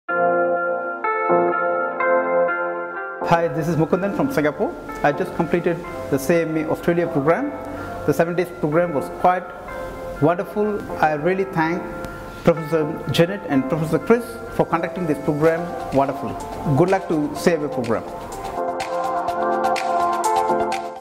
CMA AUSTRALIA- MARCH 2019 @ BANGALORE
Successfully conducted the 1st Intake of CMA program in Banglore from ICMA Australia. Great learning under Professor Janek Ratnatunga (MBA, PhD, FCA, CMA - CEO of the Institute), Dr Chris D'Souza (FCMA, PhD, FCA, FCPA).  Mr Mukundan ( chief commercial officer) from singapore